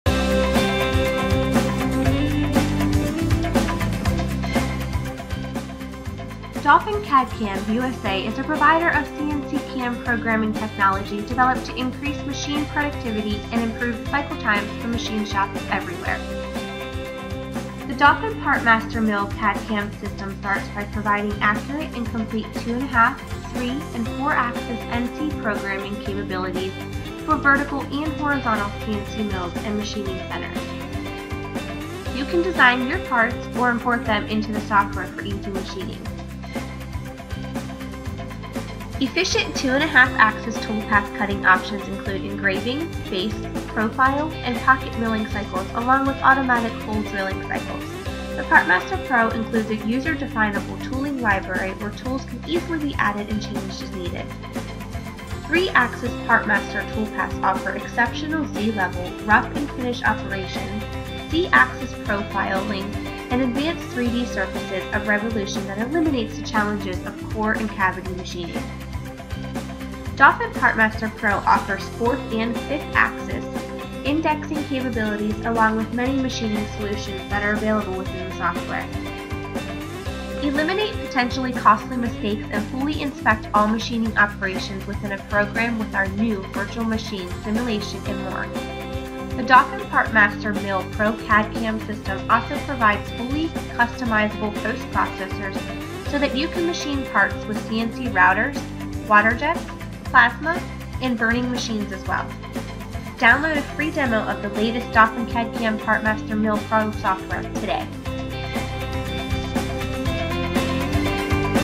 Dolphin Mill Pro Software
The Dolphin Mill Pro CNC programming software supports 2.5, 3 & 4 axis CNC Machining.  This video outlines some of the many features that Dolphin CAD-CAM Mill Pro has to offer.